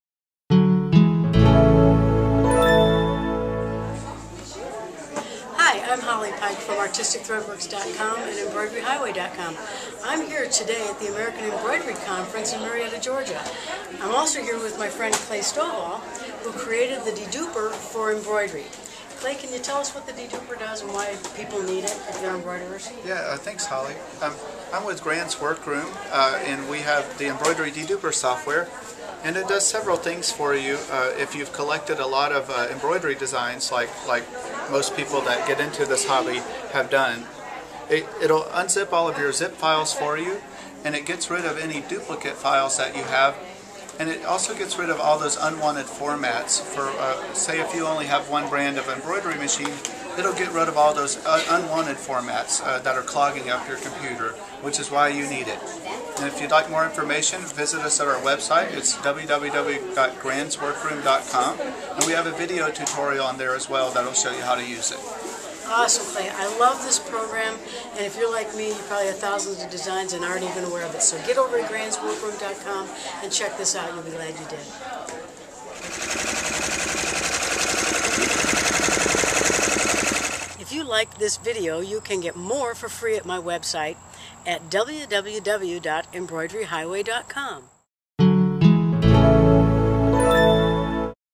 Embroidery Deduper Software w/ Holly Pike & Clay Stovall
Holly Pike talks with Clay Stovall about "DeDuper" software   DeDuper will automatically open zip files, pulling the embroidery designs out where you can use them, and remove the unwanted embroidery design formats from your hard drive.